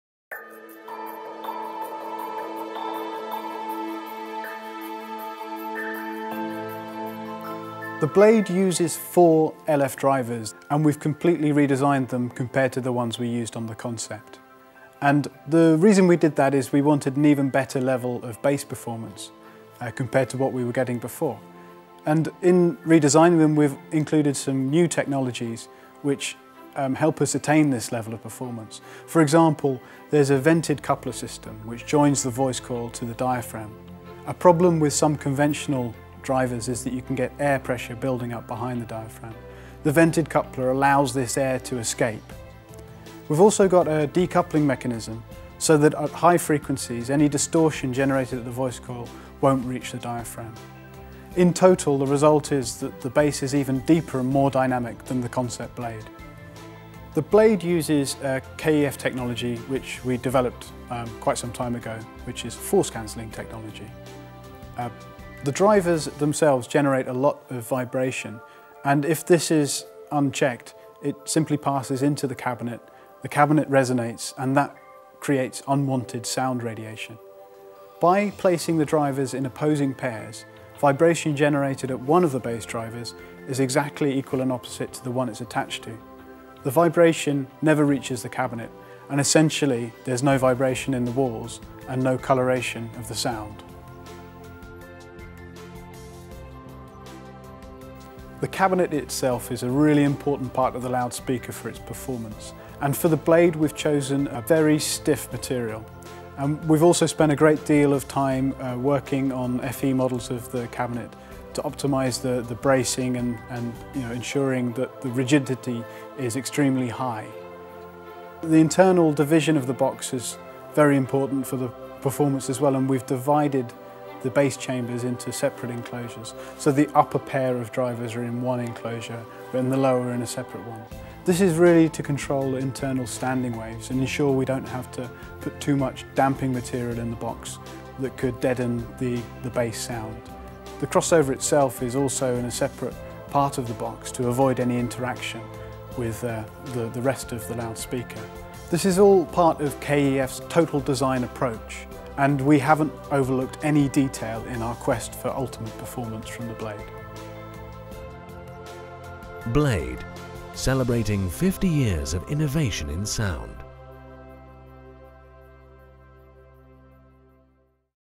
KEF Blade - Drivers and Bass Chambers
Hear KEF Acoustic Engineer, Jack Oclee-Brown explain the technologies behind the ground-breaking Blade loudspeakers - KEF's most pioneering technological advance to date.
Nothing has been overlooked in the quest for the ultimate clarity, right across the frequency range. See the technology behind the drivers and bass chambers explained here...